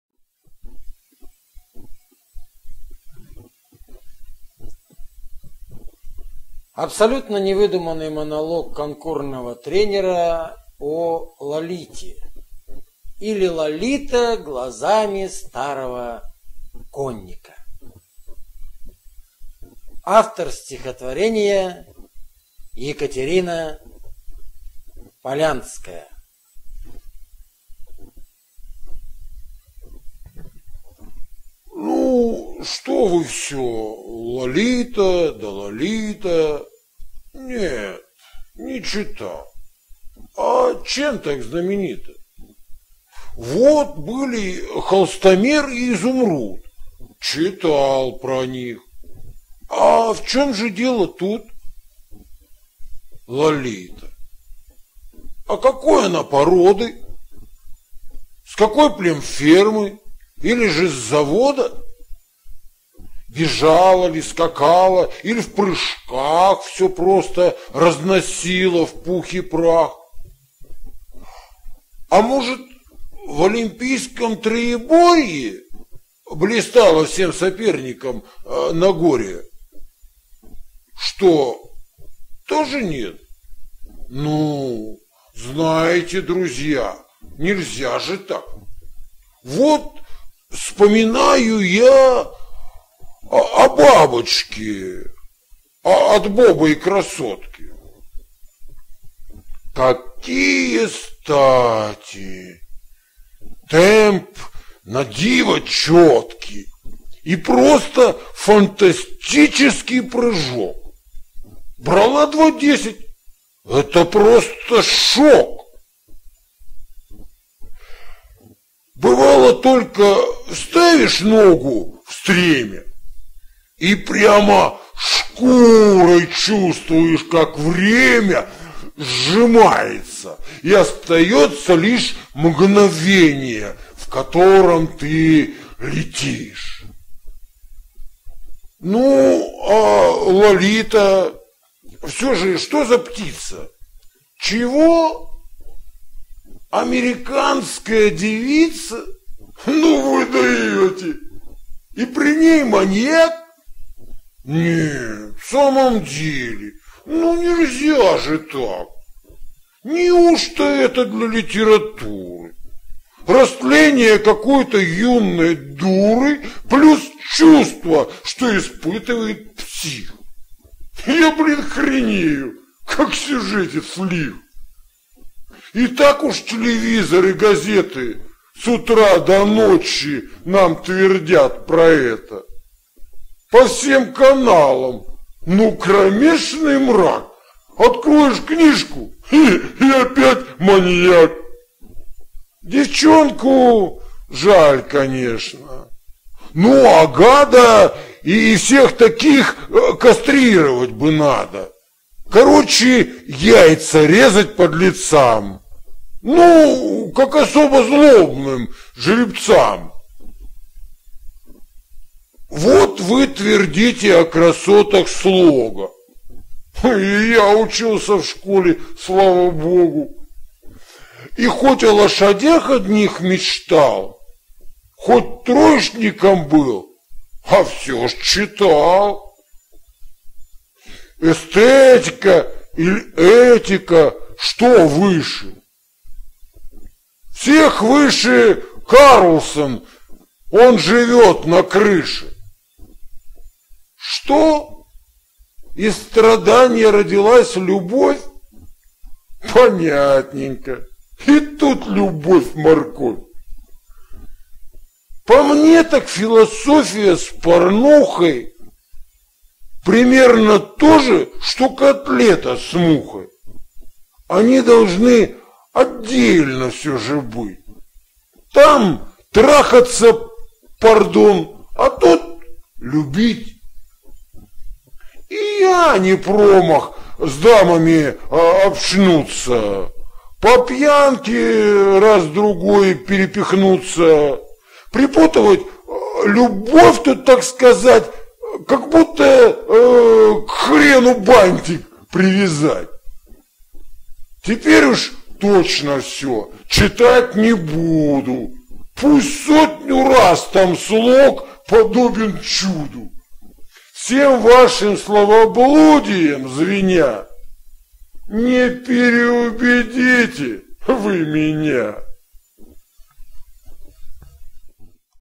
Олег Чабан - Лолита для пенсилклуба (стихи Екатерины Полянской)
Озвучили стихотворение Екатерины Полянской "Лолита для пенсилклуба".
Абсолютно невыдуманный монолог конкурного тренера о «Лолите» или «Лолита» глазами старого конника. (для пенсилклуба)

Ну что вы всё «Лолита да Лолита!» ?
Нет, не читал. А чем так знаменита?
Вот, были Холстомер и Изумруд –
Читал про них. А в чём же дело тут?
Лолита… А какой она породы?
С какой племфермы, или же – с завода?
Бежала ли, скакала, иль в прыжках
Всех просто разносила в пух и прах?
А может, в олимпийском троеборье
Блистала всем соперникам на горе?
Что, тоже – нет? Ну, знаете, друзья,
Нельзя же так! Вот, вспоминаю я
О Бабочке – от Боба и Красотки…
Какие стати! Темп на диво чёткий,
И просто фантастический прыжок:
Брала «два – десять» - это просто шок!
Бывало, только вставишь ногу в стремя,
И прямо шкурой чувствуешь, как время
Сжимается, и остаётся лишь
Мгновение, в котором ты – летишь.

Ну а Лолита, всё же, что за птица?
Чего? Американская девица?
Ну, вы даёте! И при ней – маньяк?
Нет, в самом деле, ну нельзя же так!
Неужто это для литературы:
Растление какой-то юной дуры
Плюс чувства, что испытывает псих…
Я, блин, хренею, как сюжетец лих!
И так уж телевизор и газеты
С утра до ночи нам твердят про это:
По всем каналам ну кромешный мрак!
Откроешь книжку – и опять маньяк!
Девчонку жаль, конечно. Ну а гада
И всех таких кастрировать бы надо.
Короче, яйца резать подлецам,
Ну, как особо злобным жеребцам.

Вот вы твердите о красотах слога.
И я учился в школе, слава Богу,
И хоть о лошадях одних мечтал,
Хоть троечником был, а всё ж – читал.
«Эстетика, иль этика – что выше?» -
Всех выше Карлсон- он живёт на крыше.
Что? Из страданья родилась любовь?
Понятненько: и тут любовь-морковь.
По мне, так философия с порнухой
Примерно то же, что котлета с мухой.
Они должны отдельно, всё же, быть:
Там – трахаться, пардон, а тут – любить.
И я не промах с дамами общнуться,
По пьянке раз-другой перепихнуться…
Припутывать любовь тут, так сказать,
Как будто к хрену бантик привязать.
Теперь уж точно всё. Читать не буду.
Пусть сотню раз там слог подобен чуду!
Всем вашим словоблудием звеня,
Не переубедите вы меня!